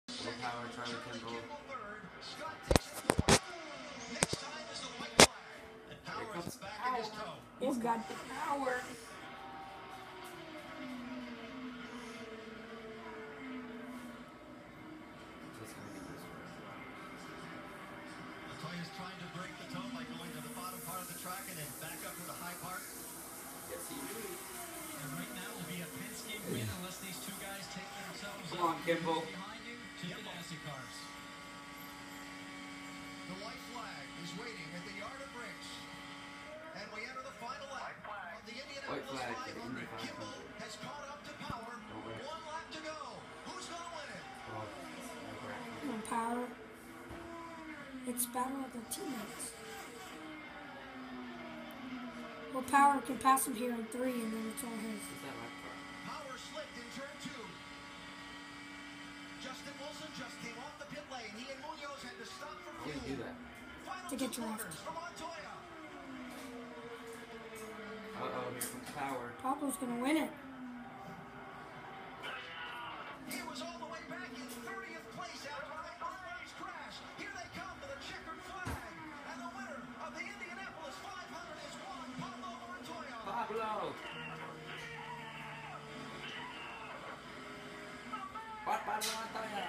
Juan Pablo Montoya Wins The 2015 99th Indianapolis 500!!!!!!!
Yay Montoya Won!